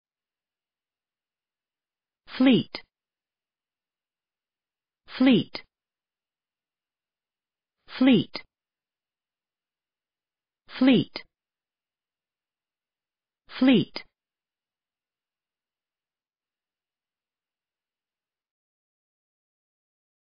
How to pronounce fleet
fleet American English pronunciation. How to pronounce fleet correctly. How to say fleet in proper American English.